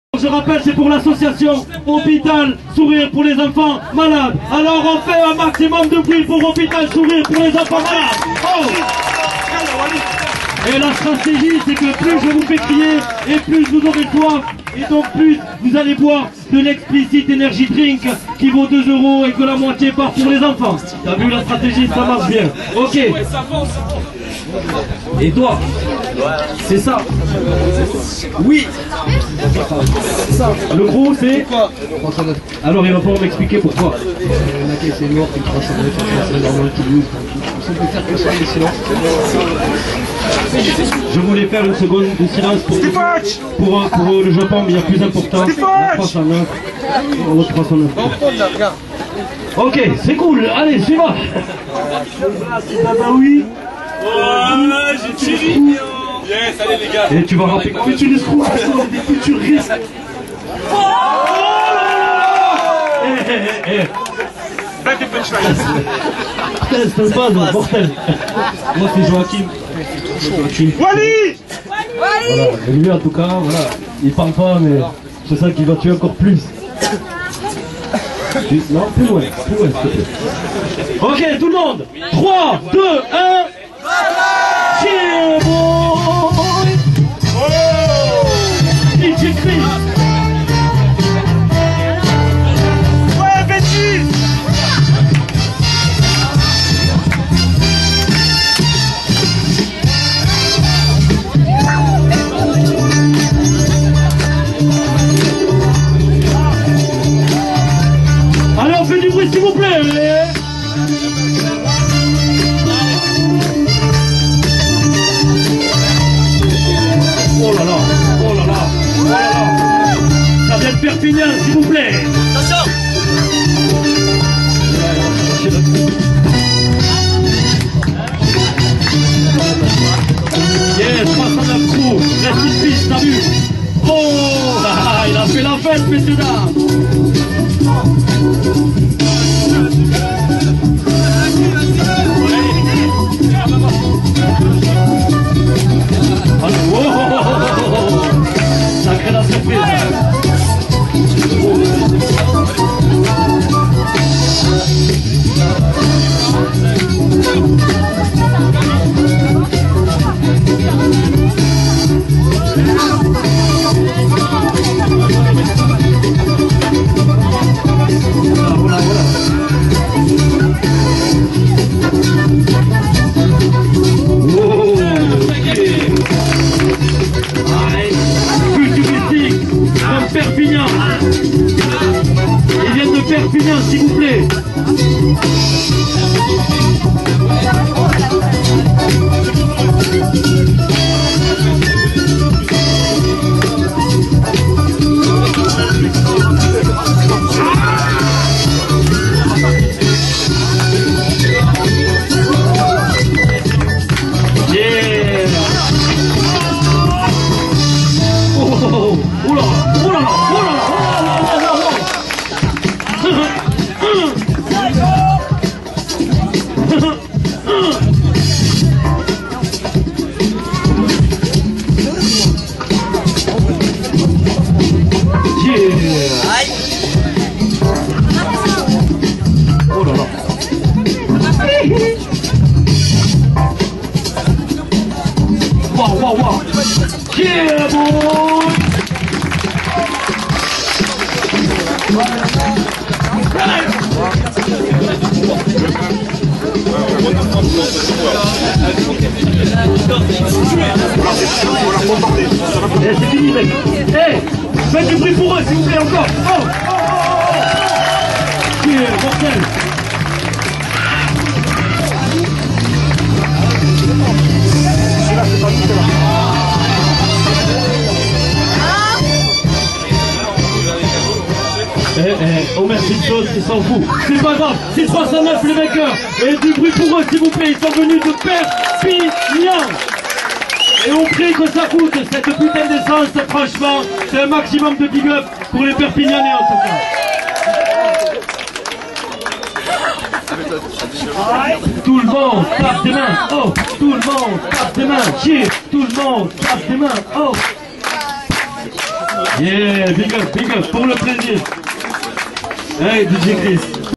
futurisk vs 309 crew
battle solidarité place de l'europe a toulouse